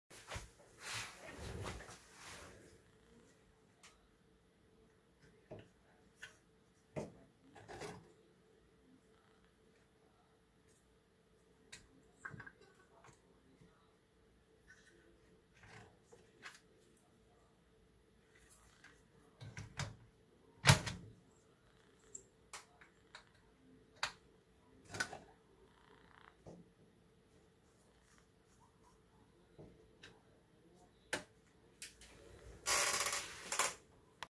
Mounting CNC Dust Collector to Snapmaker2.0.
This item is installed to remove dust generated during CNC work. It is easy to install and remove, and the rotation function that responds to movement is outstanding.